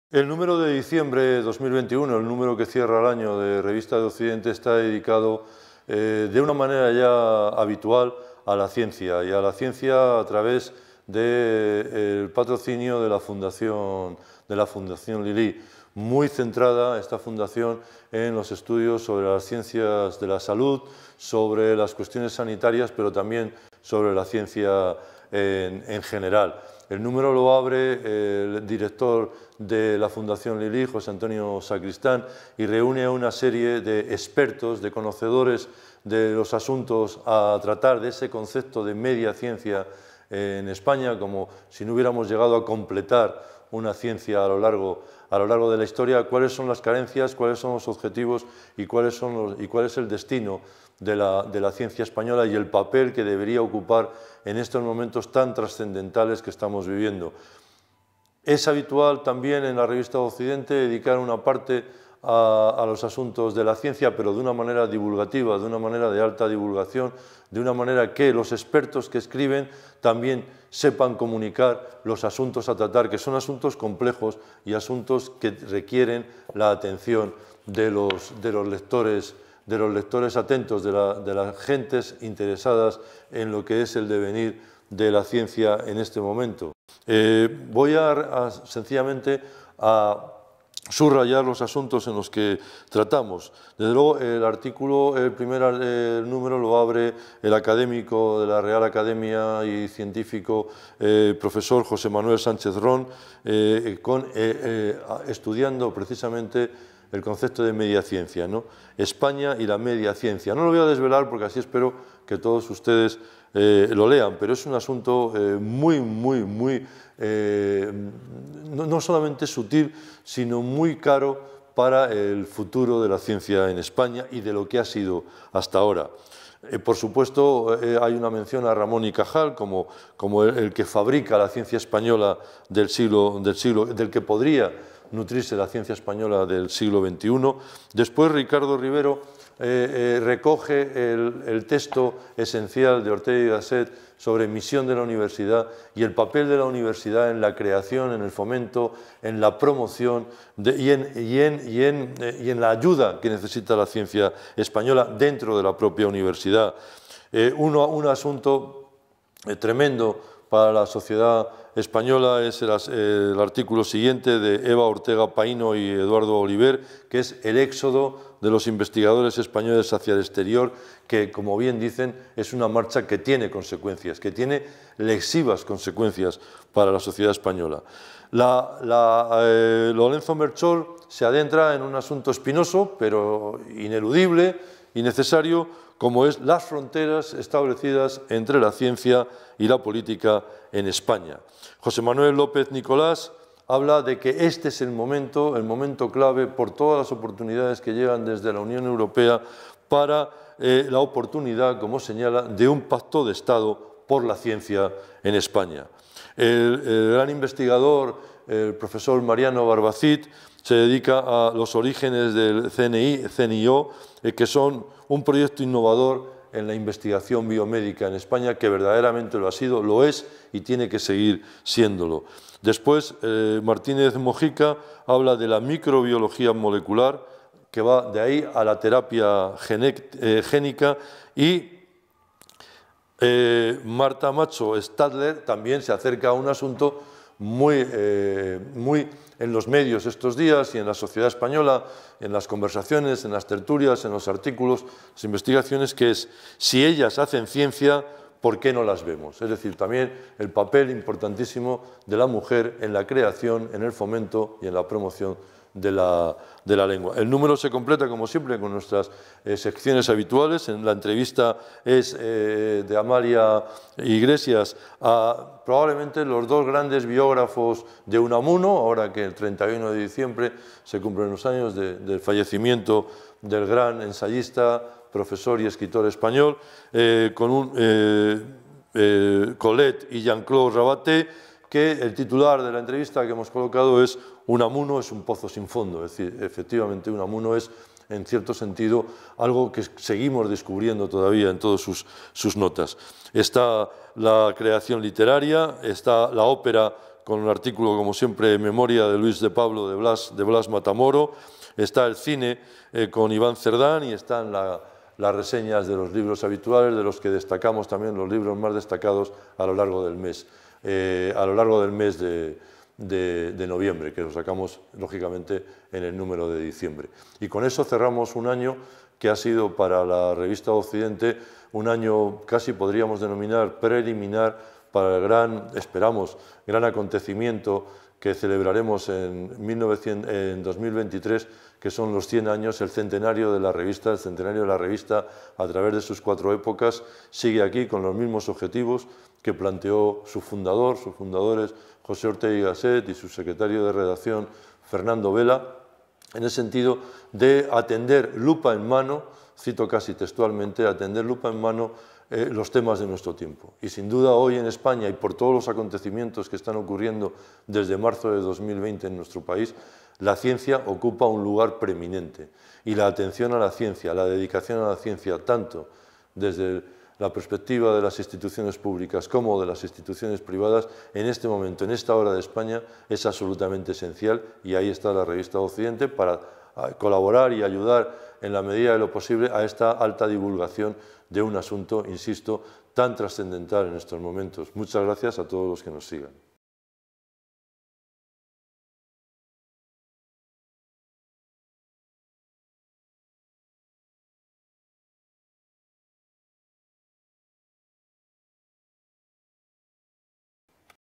Revista de Occidente: España y la media ciencia
Entrevista a Fernando R. Lafuente, secretario de redacción de Revista de Occidente, para conocer los contenidos del nº 487 (diciembre).